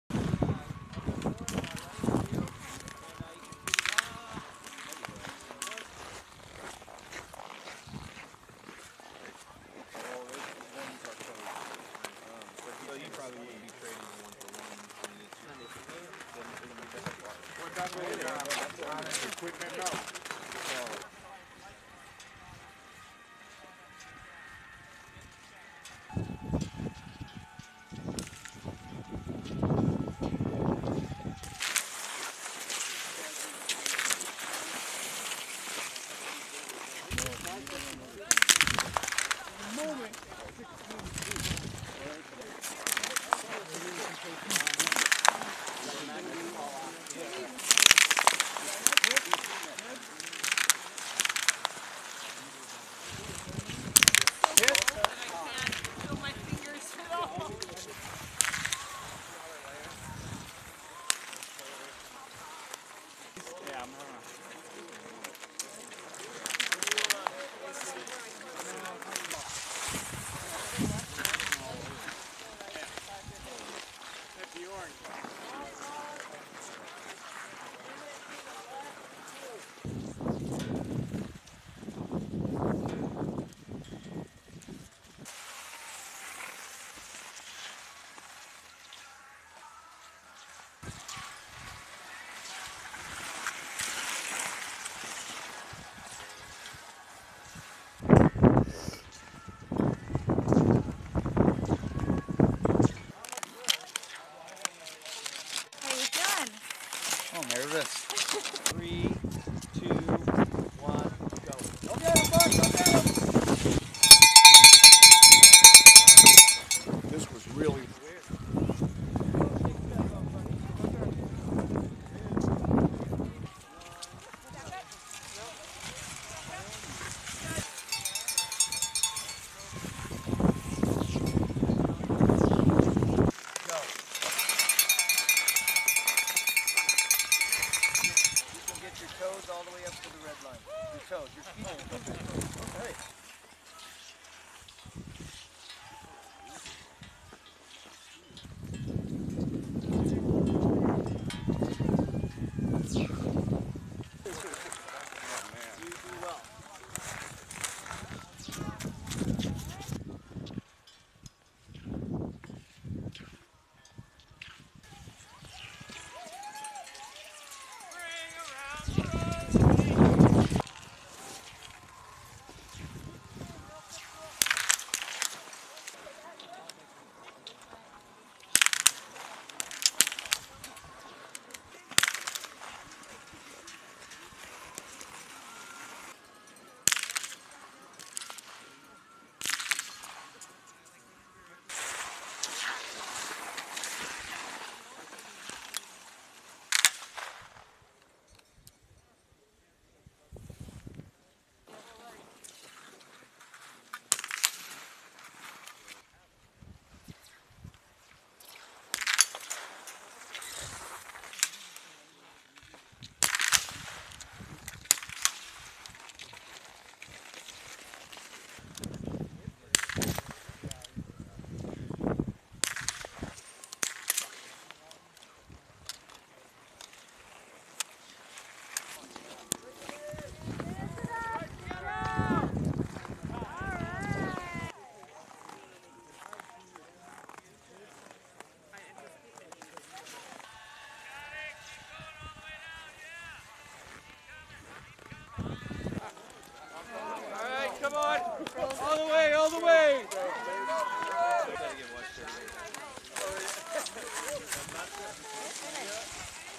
2022 CNGB Biathlon Championship: Sprint Race
B-Roll of the CNGB Biathlon Championships individual Sprint race at Camp Ripley, February 13, 2022.

2022 CNGB Biathlon Championship: Sprint Race
LITTLE FALLS, MN, UNITED STATES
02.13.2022

Film Credits: Video by Sgt. Mahsima Alkamooneh
Camp Ripley Training Center